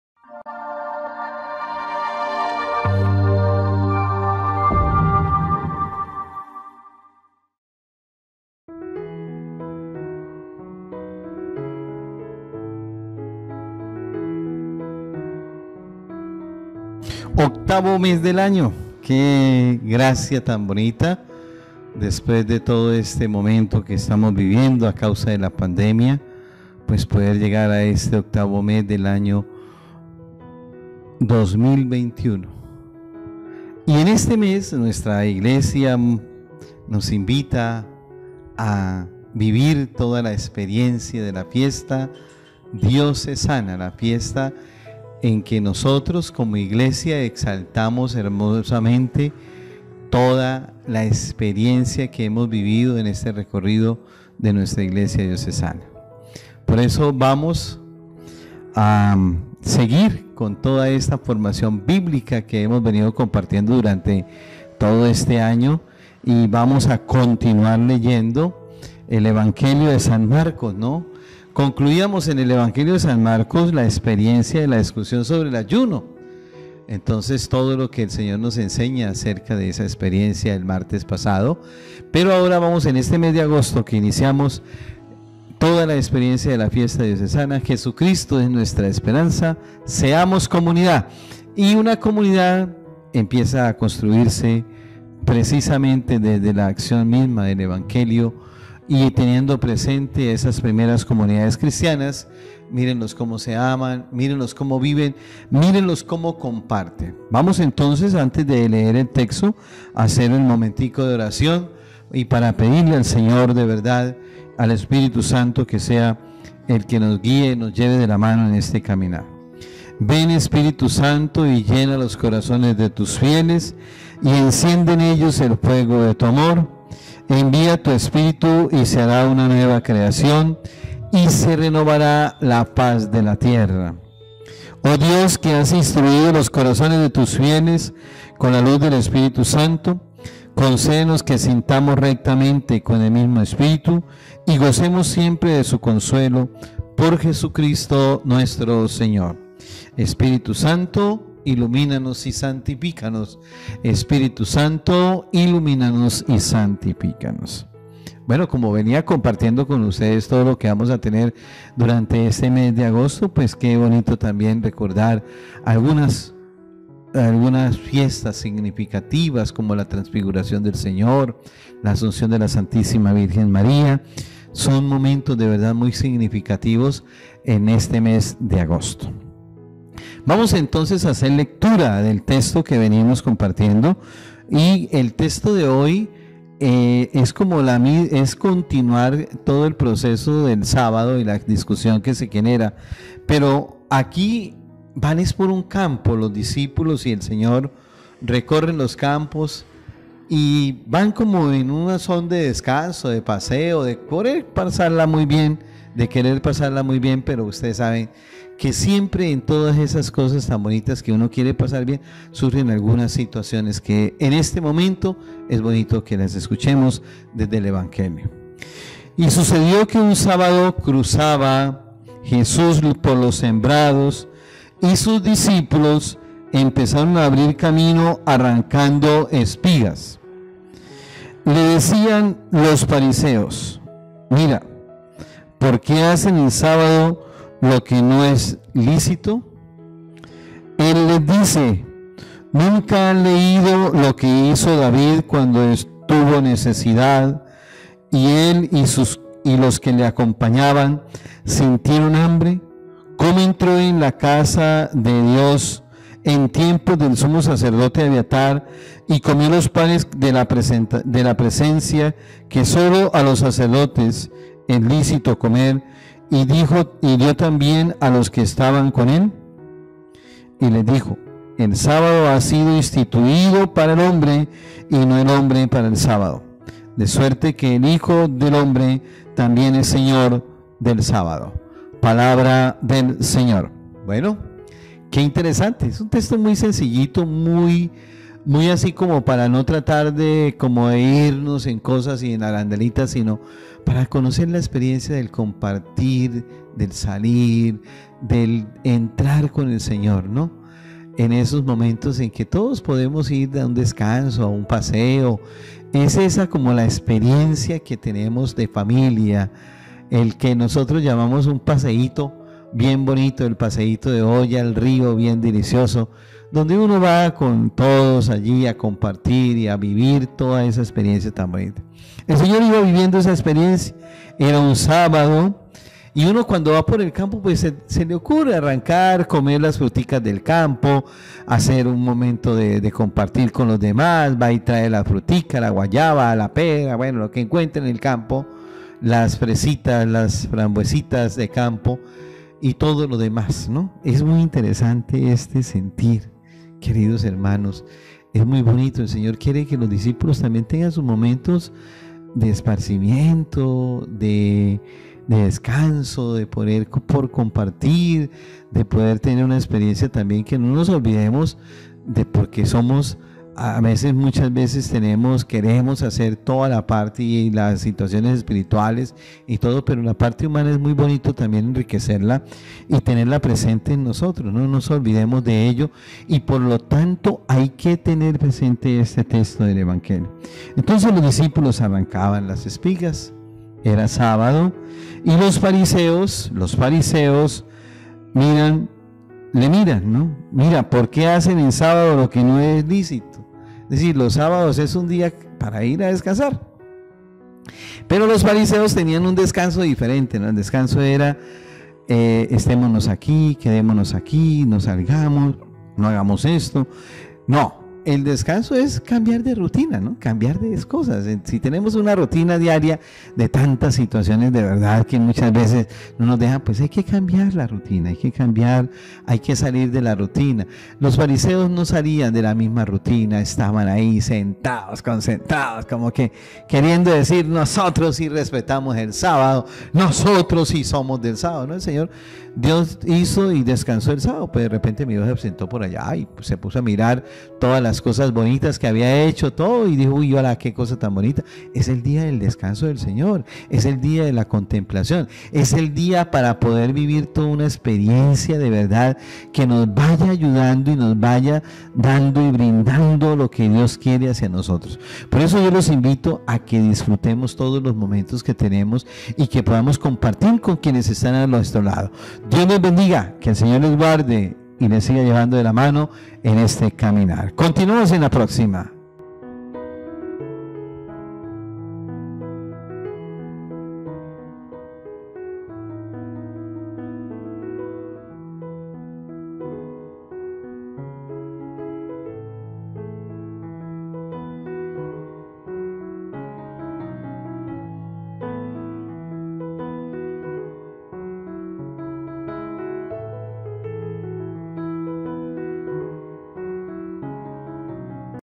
𝗙𝗼𝗿𝗺𝗮𝗰𝗶𝗼́𝗻 𝗕𝗶́𝗯𝗹𝗶𝗰𝗮 | 1 de agosto, 2021
El Señor se ha manifestado a nosotros a través de su palabra, vengan, volvamos a Él, compartamos unos minutos con nuestro programa todos los martes y jueves, dirige el Pbro. Cesar Prato.